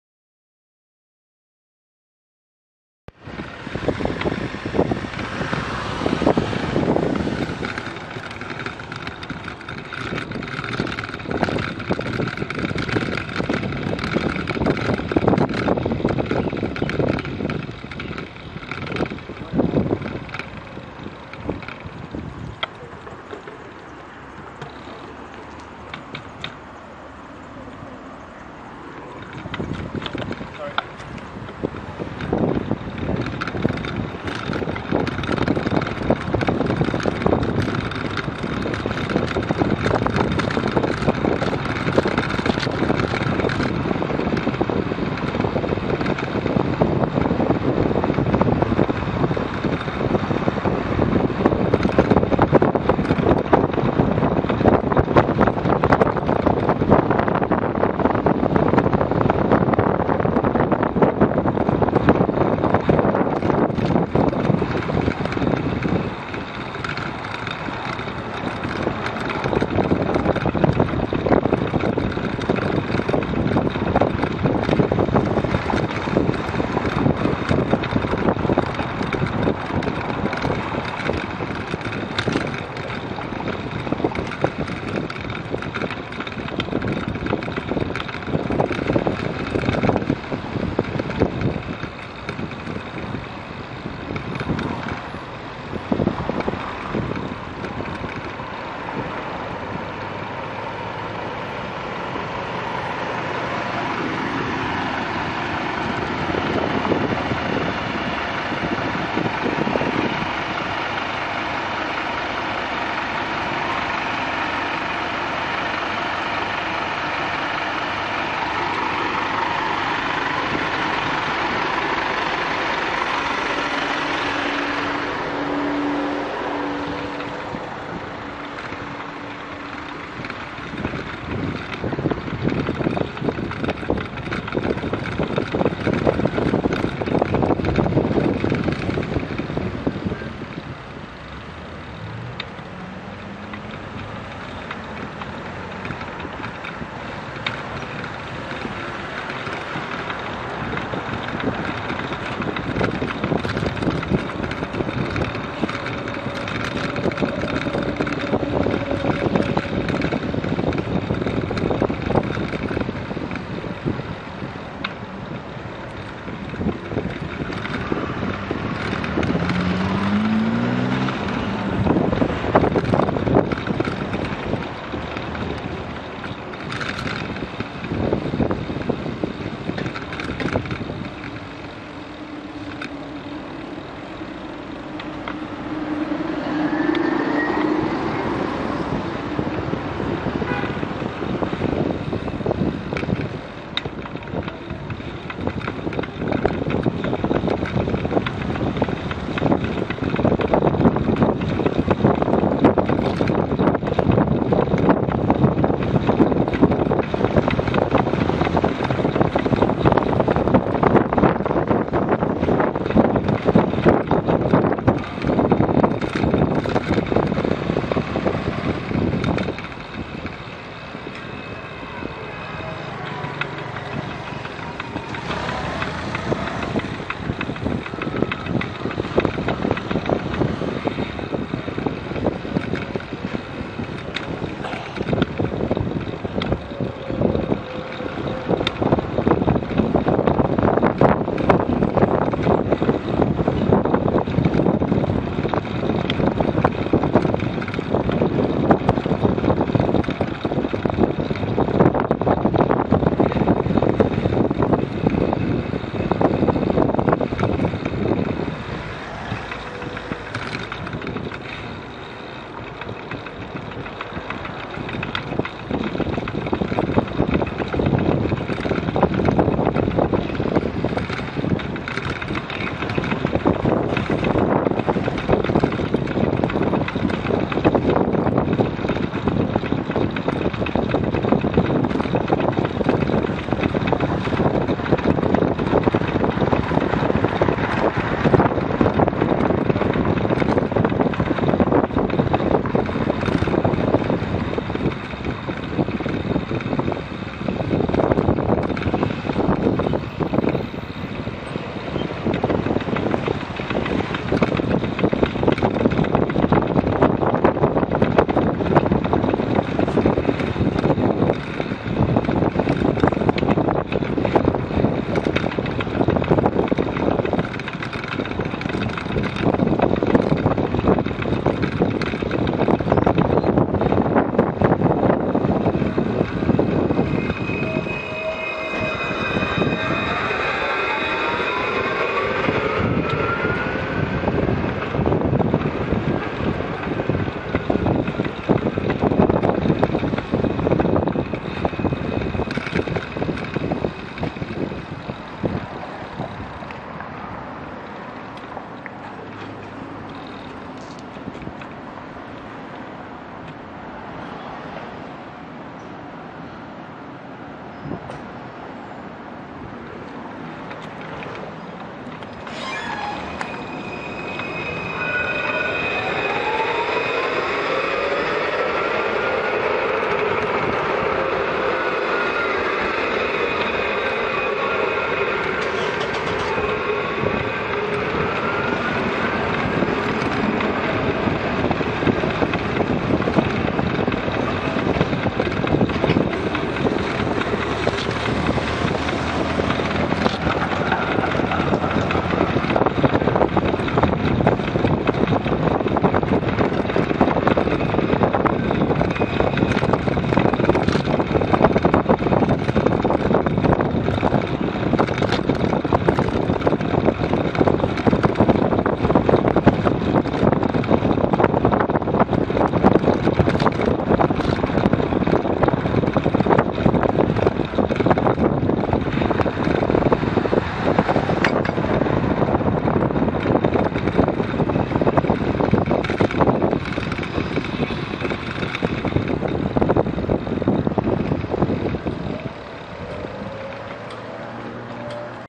Victoria Pde and Victoria St (Smith St to River Blvd)
Mar 03, '16, 03:49PM

This video provides evidence for updating and improving Google Maps Cycling layer.